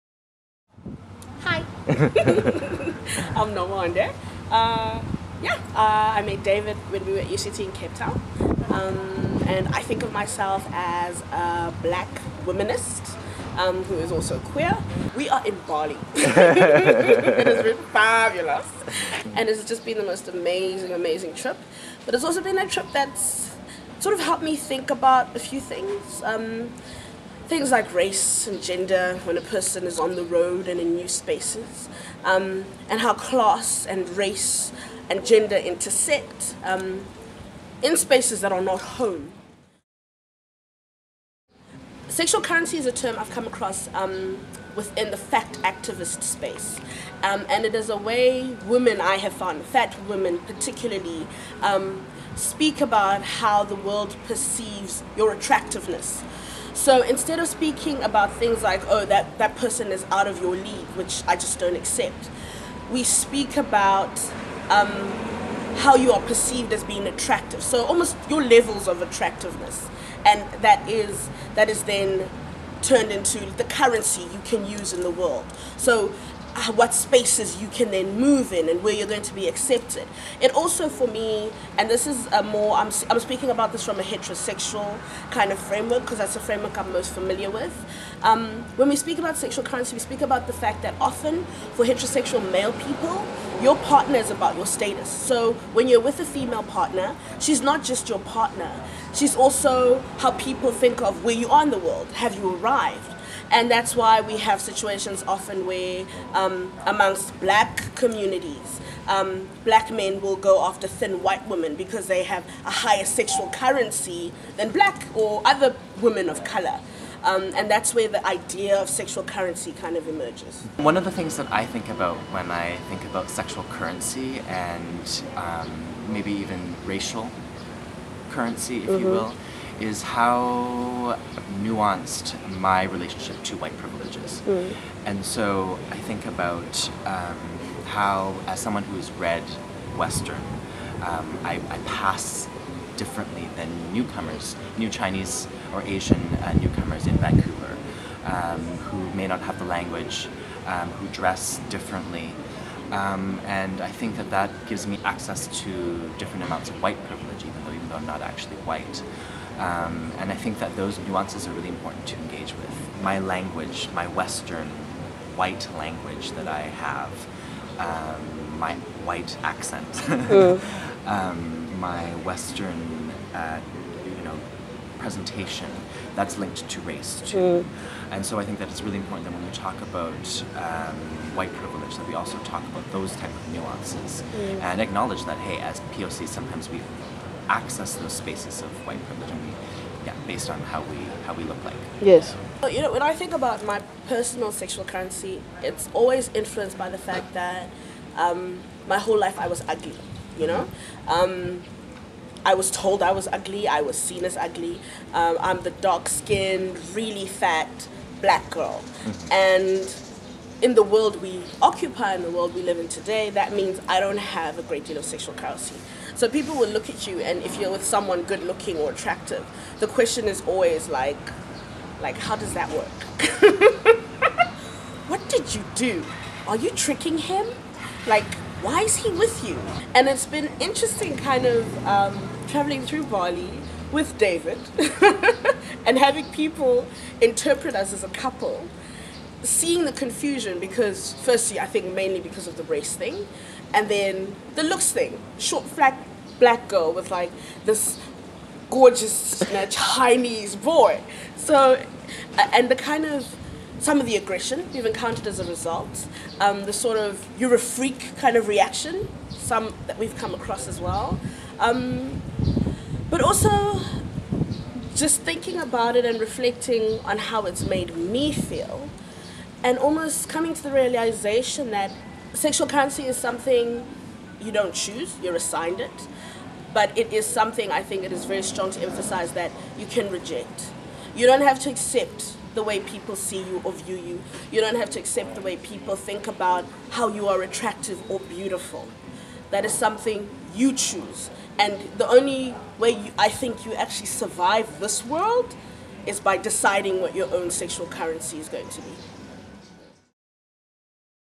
The Politics of Sexy
David and Nomonde explore the intricate complexities of desirability and its links to race and privilege.